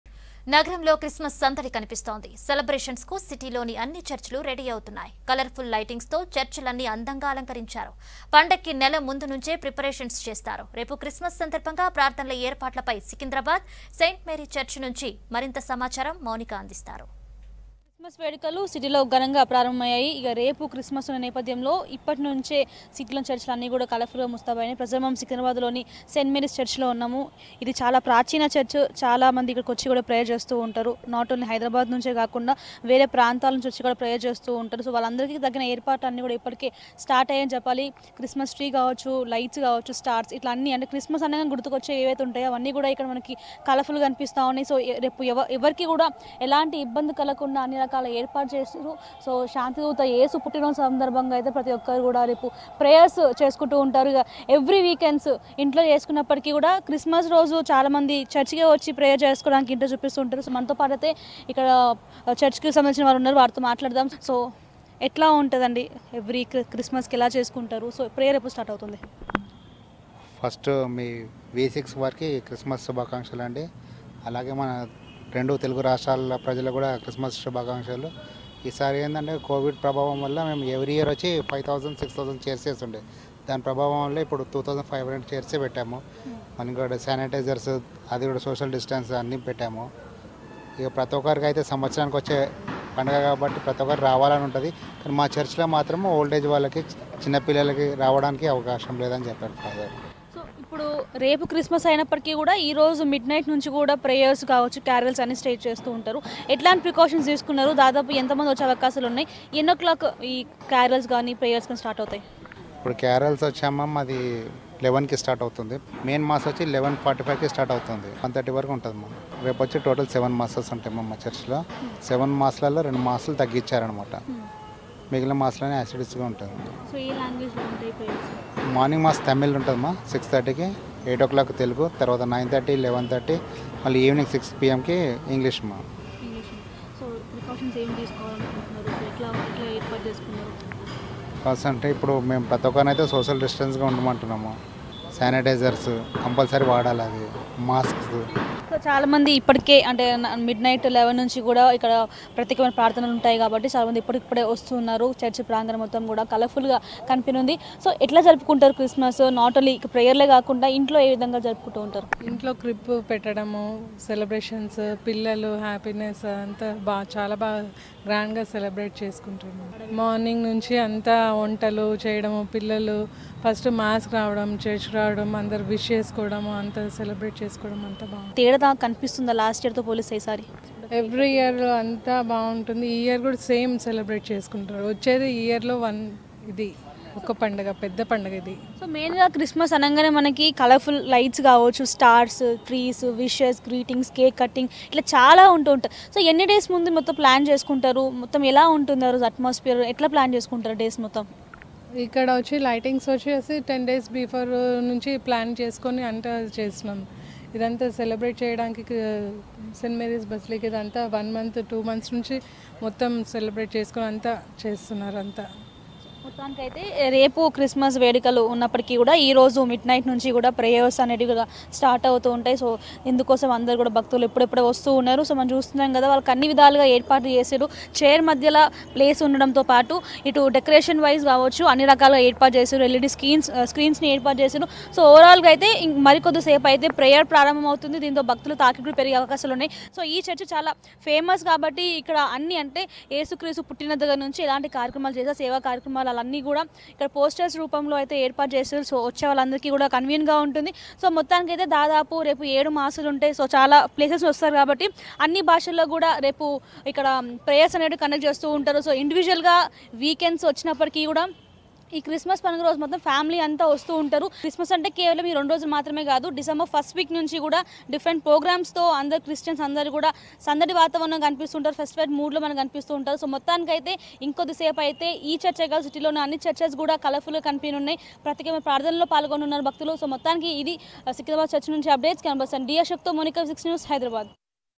All Set For Christmas Celebrations In Secunderabad St. Mary Church | V6 News
All Set For Christmas Celebrations In Secunderabad St. Mary Church.